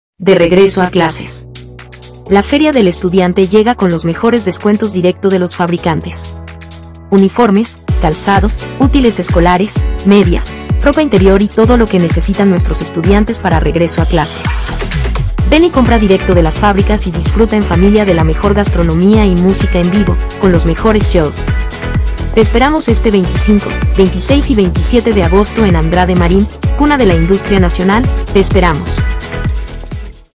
Feria del Estudiante de regreso a clases 2023 Andrade Marín
De regreso a clases...👉LA FERIA DEL ESTUDIANTE 2023...llega con los mejores descuentos directo de los fabricantes...
✅ Uniformes,
✅ Calzado,
✅ Útiles Escolares,
✅ Medias, Ropa Interior y todo lo que necesitan nuestros estudiantes para regreso a clases
🚩 Ven y compra directo de las fábricas y disfruta en familia
🟢 La mejor gastronomía y los mejores shows musicales